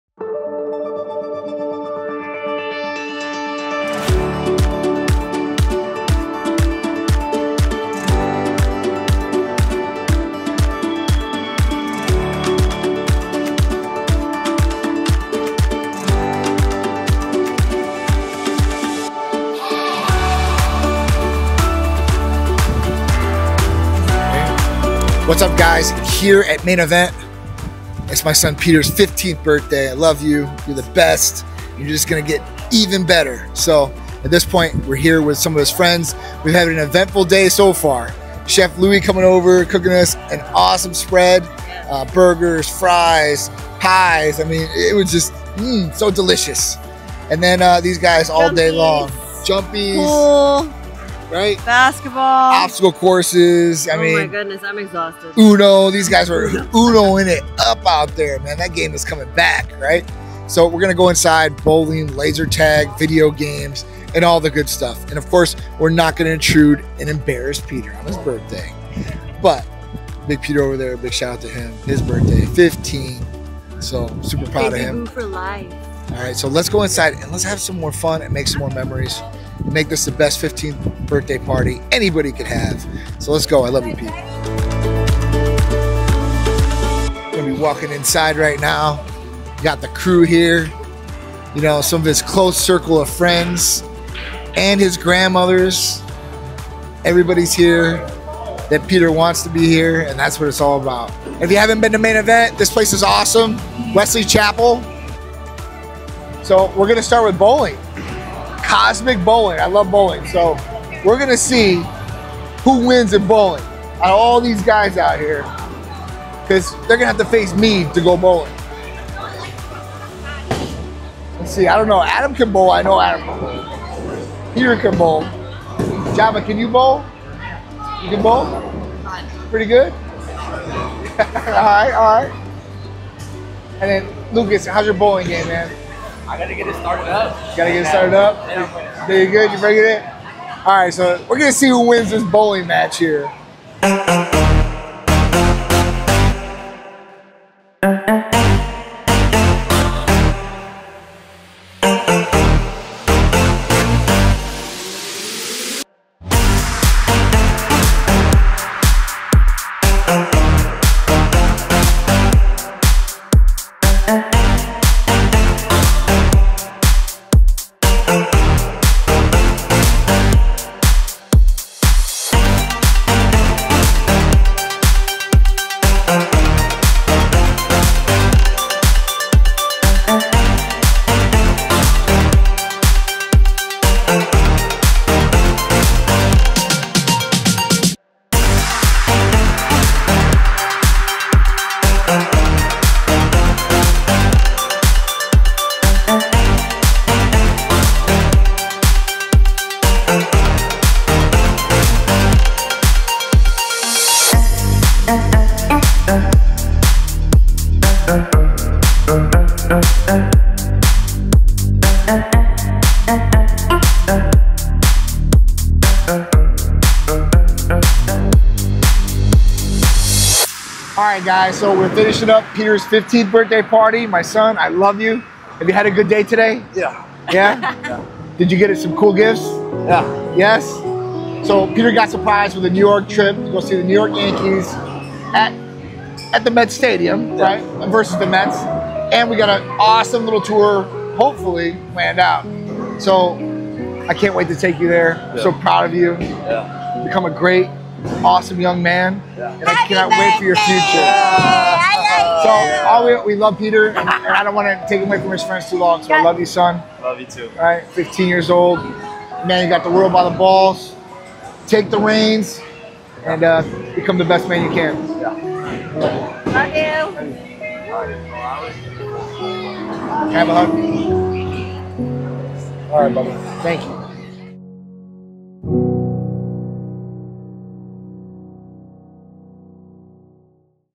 TitanMedical owners celebrate the 15th Birthday of their son Peter Tsikouris!
Happy birthday Peter! John & Sharisse helped Peter celebrate his birthday at Main Event and had a great time with friends and family!